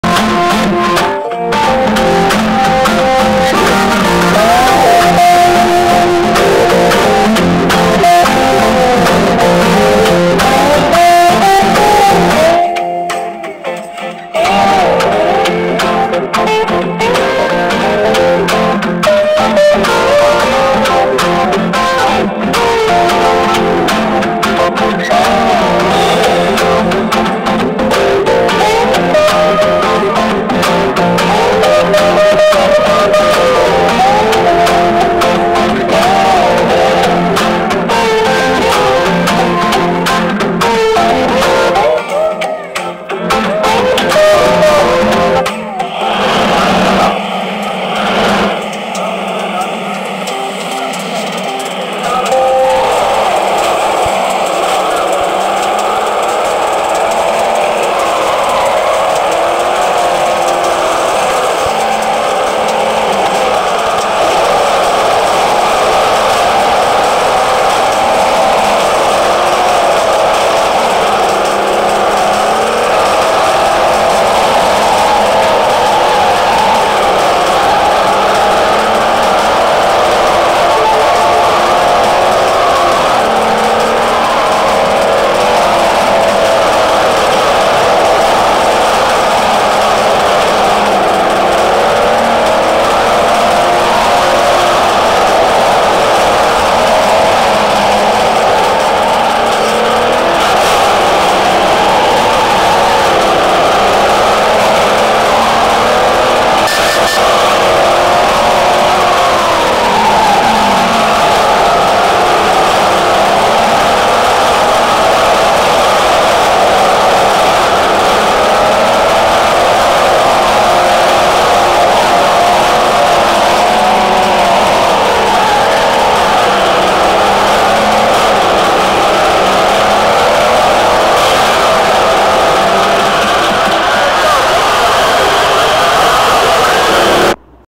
The Freedom Factory's 2nd off-road Race - Bird Eye View - Lampooner brown car - the Boomerang track
Here's our second off-road race from the freedom factory. It's a great first experience having a bird eye view of this off-road race. Selected the lampooner brown car on this Boomerang off-road race track, unfortunately resulted 3rd place at the end.